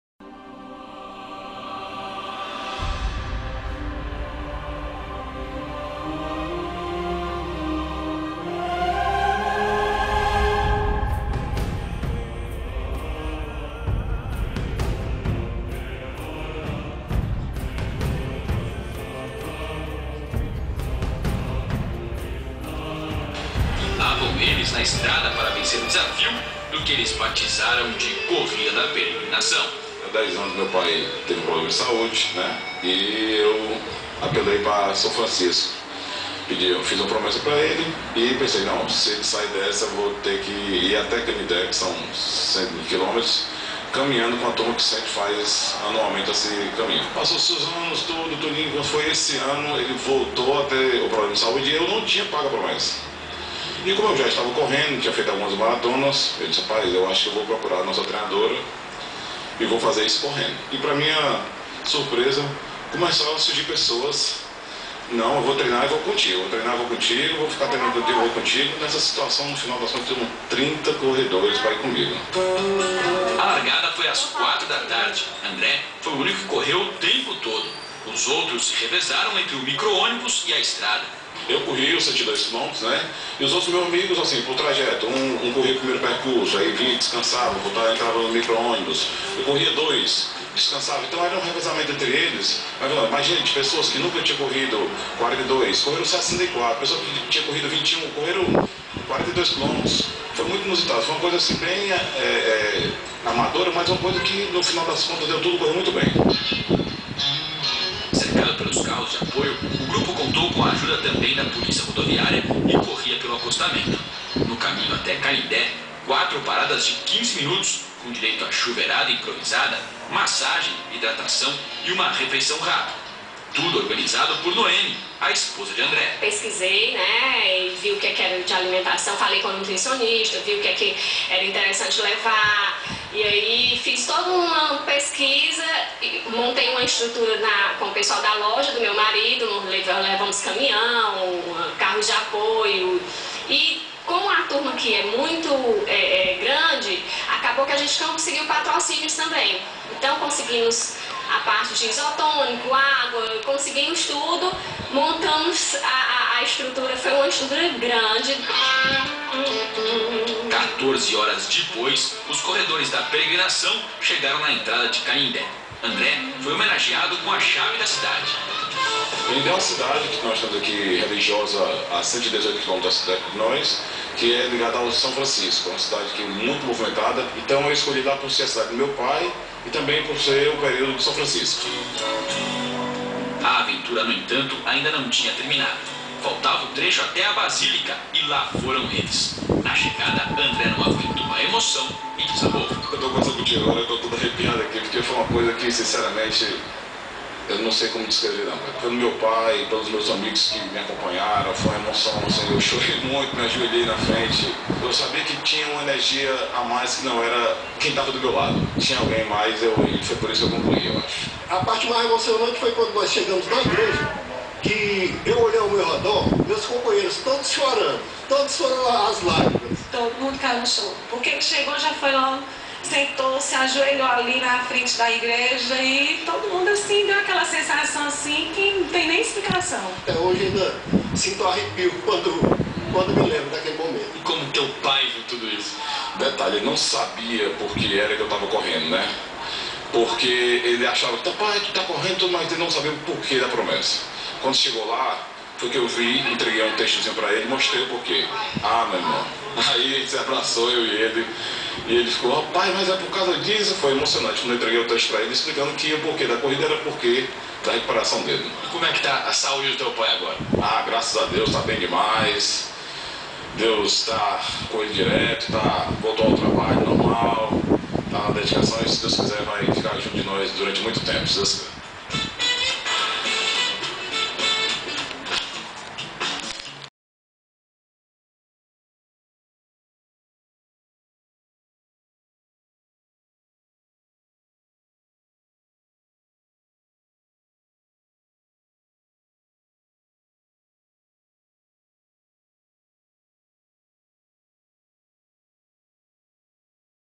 Corrida da Peregrinação no ESPN.
Uma linda história de fé e coragem que fez esse grupo de amigos , alunos e professores se unirem em um único objetivo: Correr 100KL.
Foi a Corrida da Peregrinação que teve destaque no canal ESPN, confiram!
OBS: Este vídeo foi gravado do canal ESPN com uma câmera simples  e sem nenhum interesse além de divulgar esse linda história!